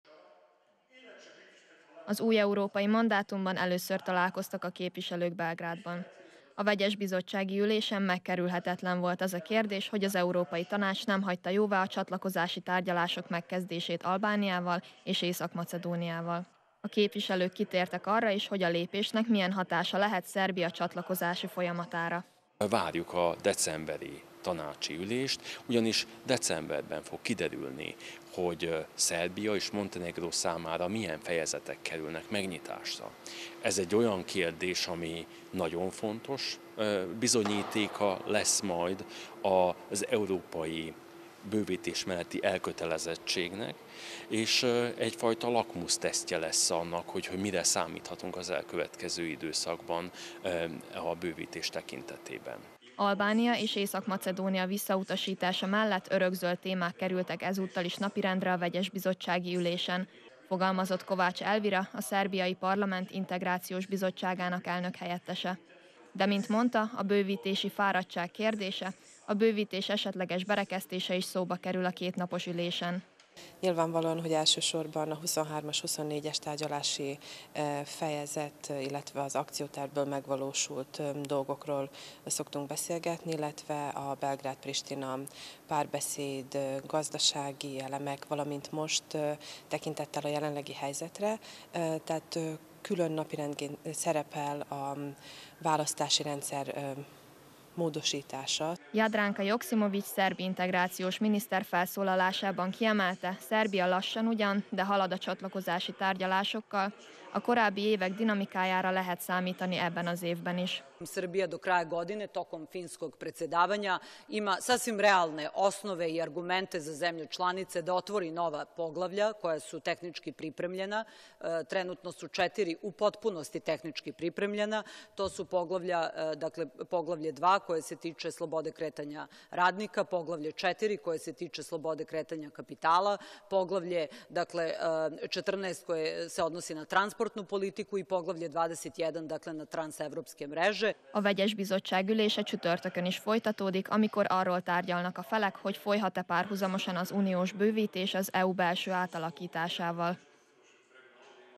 2019.10.30. - Megkezdődött az EP és a Szerbiai Képviselőház vegyesbizottságának kétnapos ülése
Belgrádban tartják az Európai Parlament és a Szerbiai Képviselőház vegyesbizottságának kétnapos ülését.